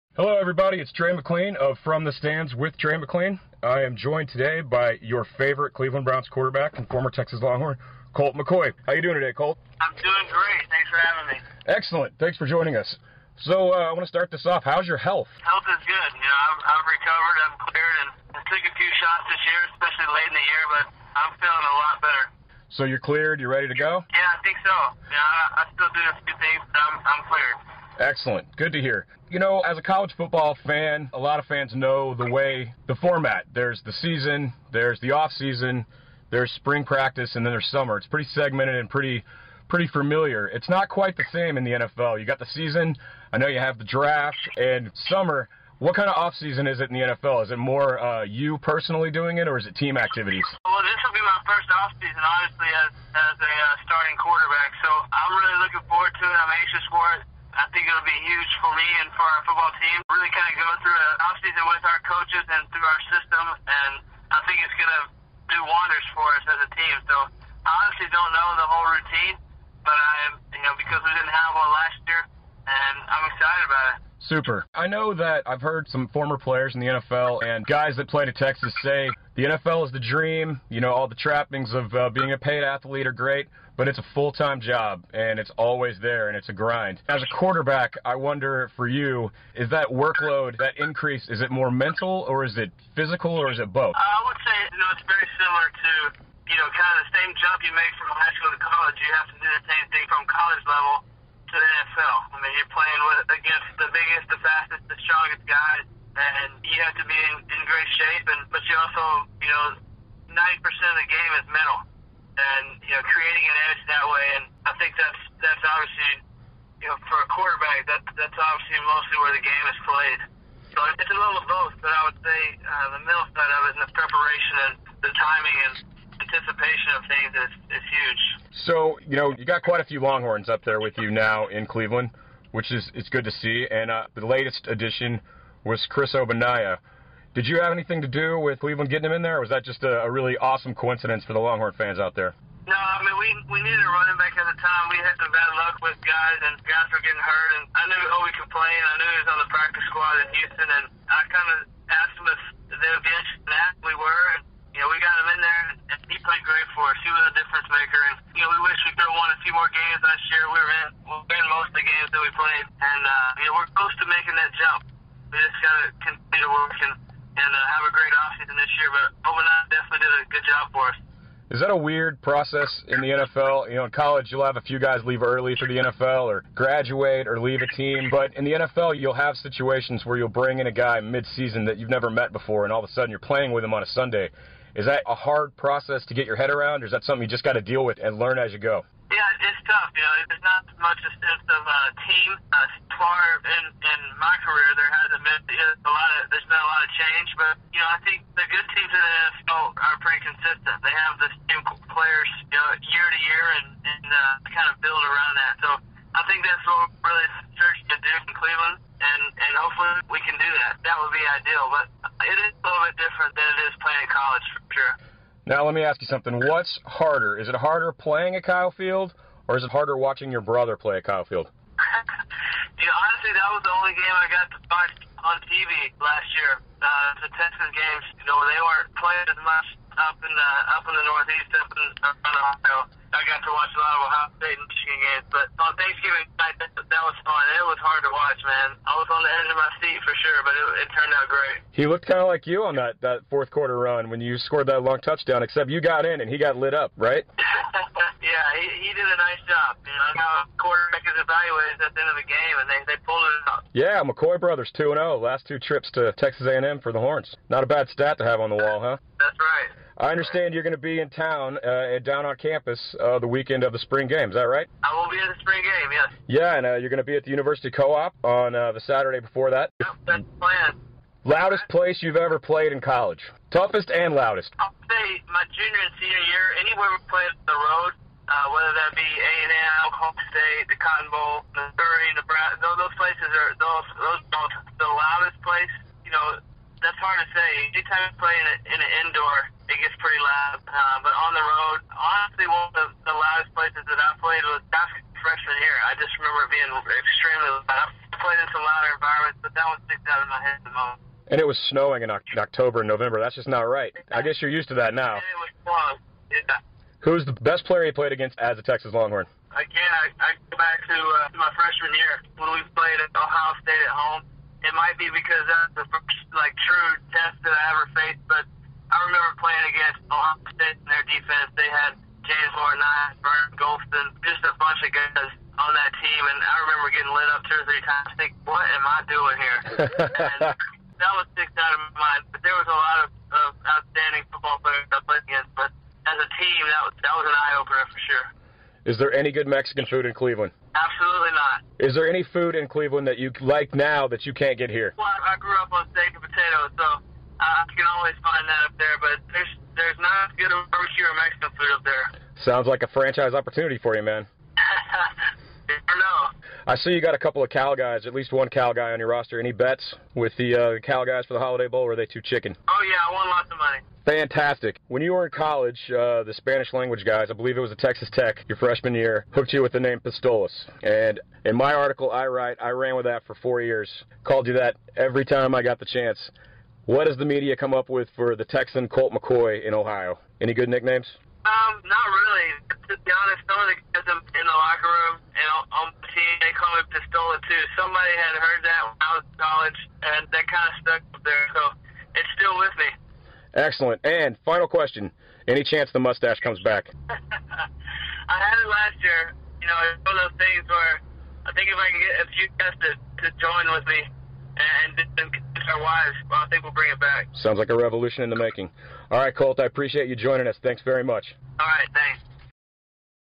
Colt McCoy interview 3/20/12
Trey McLean of "From The Stands" catches up with Colt McCoy!

The University Co-op has great Longhorn gear for everyone in your family!